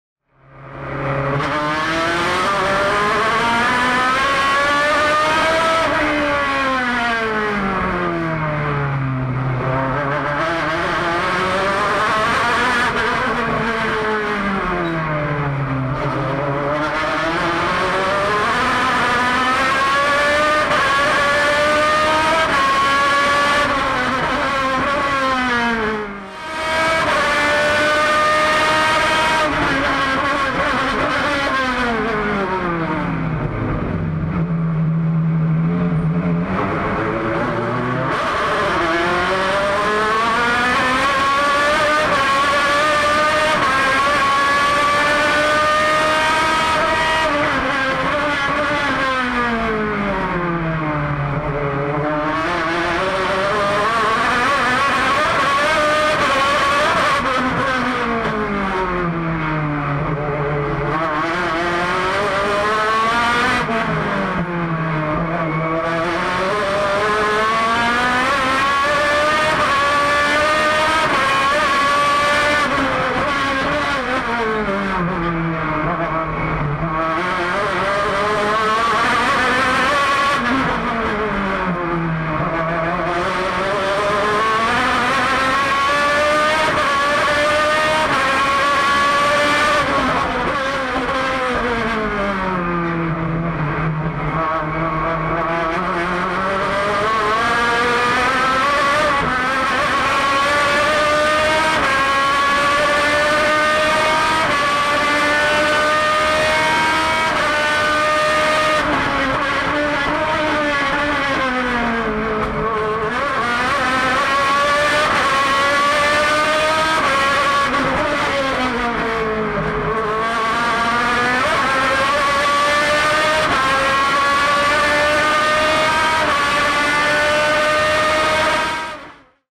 La largada nocturna de Franco Vivian en Santa Fe
Sentí la sensación de manejar de noche el Chevrolet Cruze del EQUIPO YPF CHEVROLET de STC2000 en el trazado callejero santafesino.